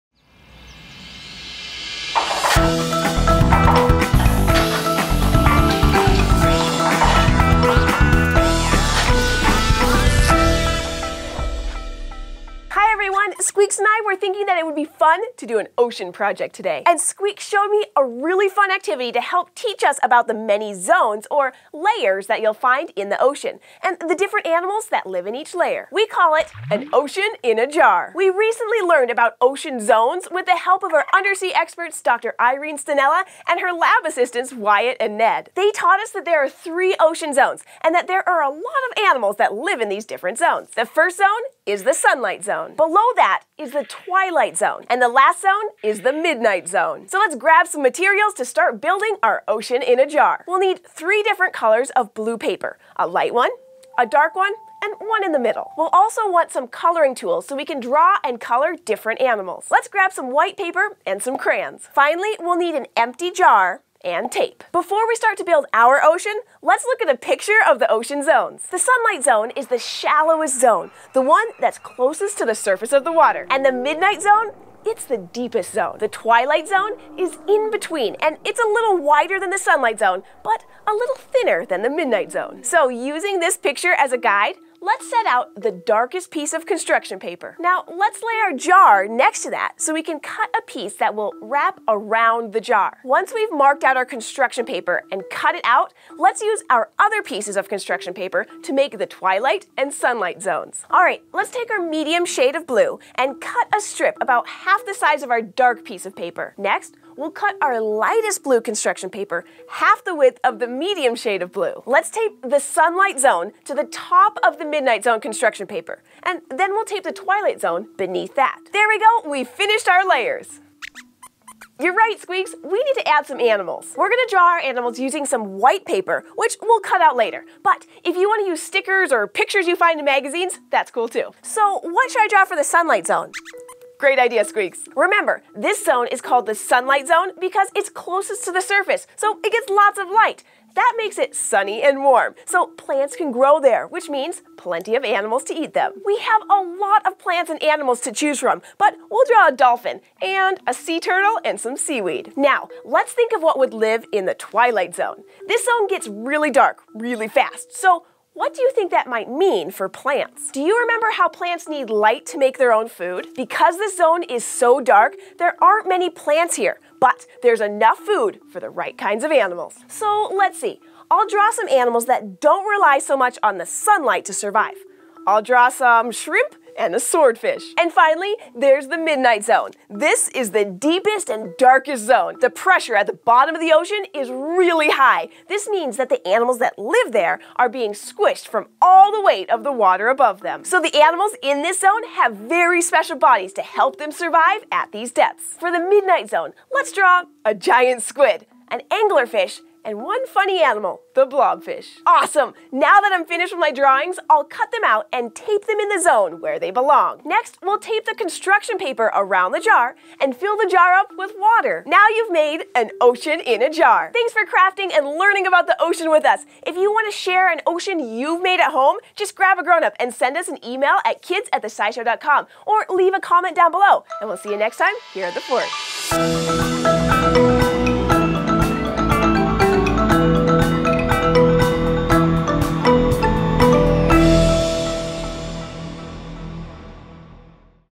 Make the Ocean in a Jar!
We've been learning a whole lot about the ocean lately, so we thought it might be fun to put all that knowledge to use with a fun project! Join us as we make an ocean in a jar and learn a little more about the different ocean zones!